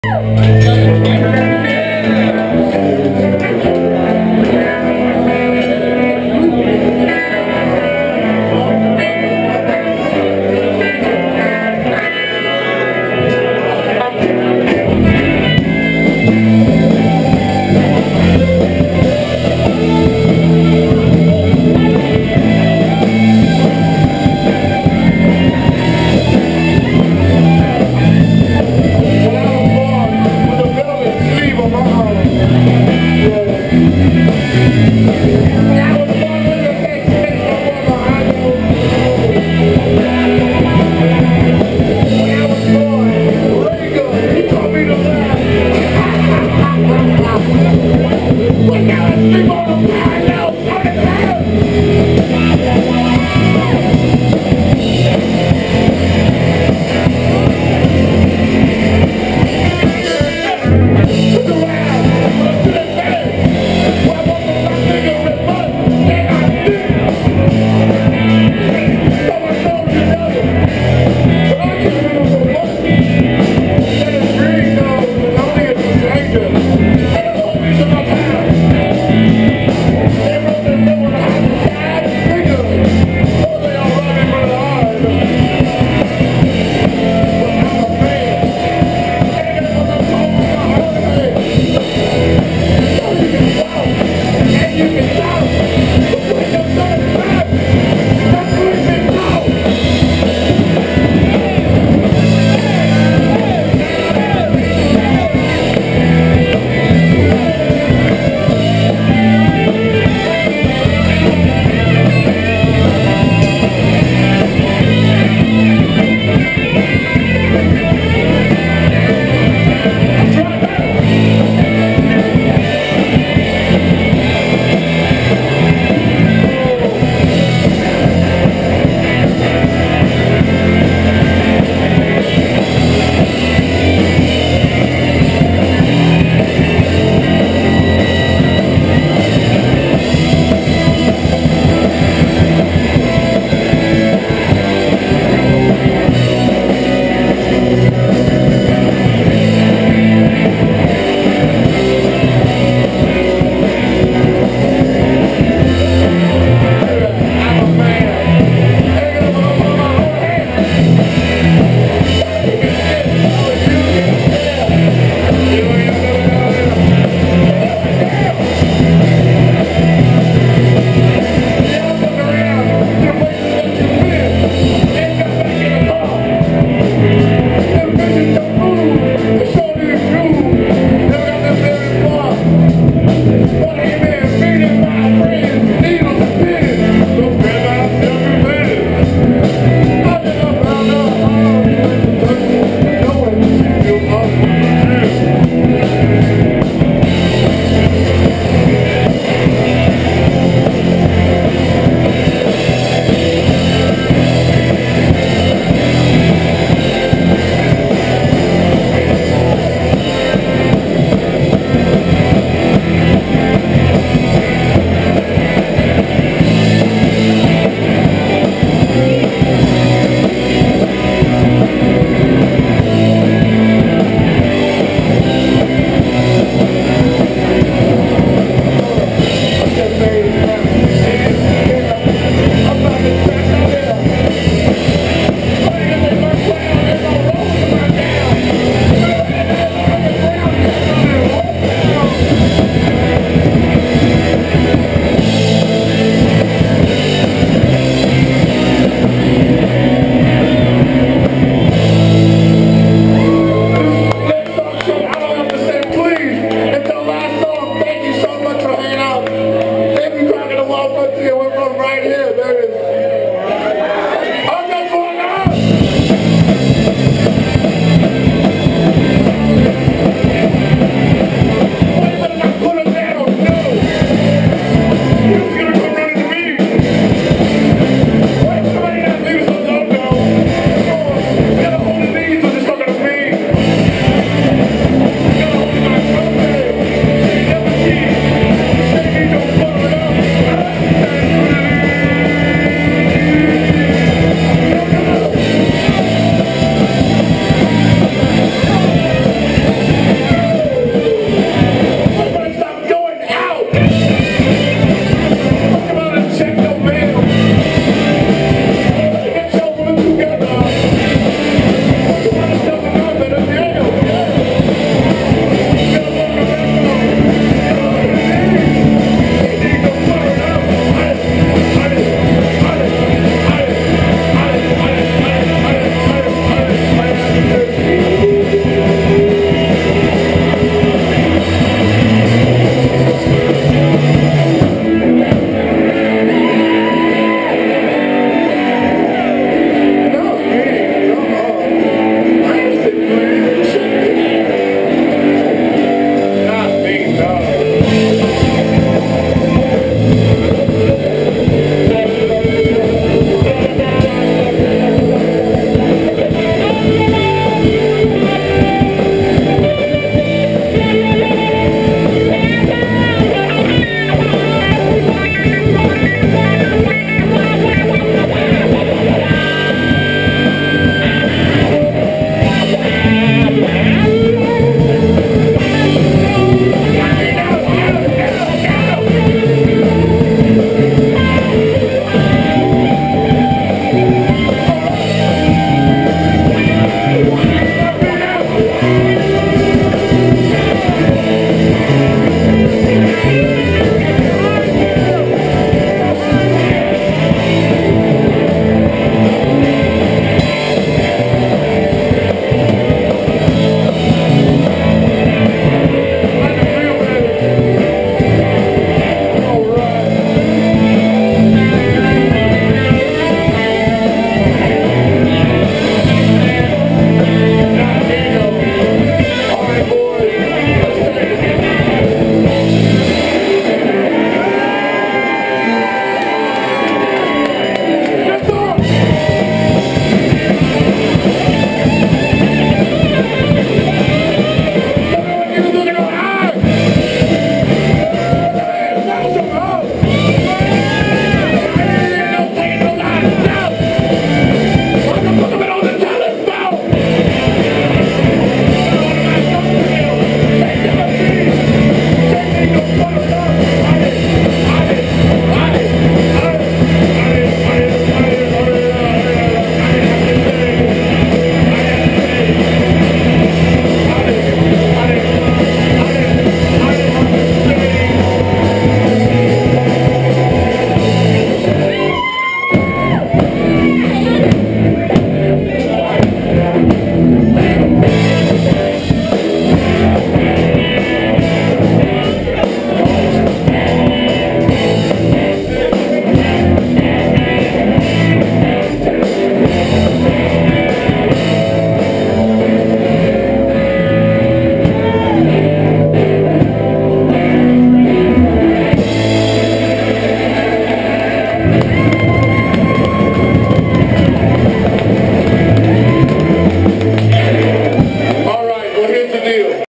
Davy Crockett and the Wild Frontier LIVE at Tugy's on its last night in business
After over 100 years in business serving North Shore patrons, Tugendaft's Tavern in Covington closed its doors, but not before Davy Crockett and the Wild Frontier had its way with it.  Seen only here, the misunderstood faces of modern rock 'n' roll music making sweet love to a beloved and sorely missed Louisiana landmark.